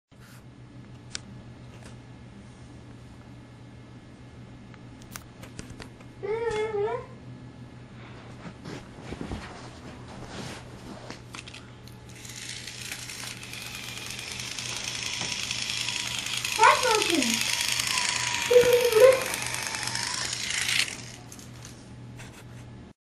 Thomas the train travels past friends in yard
Uncut video of Thomas the tank engine going past his friends in the yard. Trackmaster toys. Shot in high definition on canon sx150is.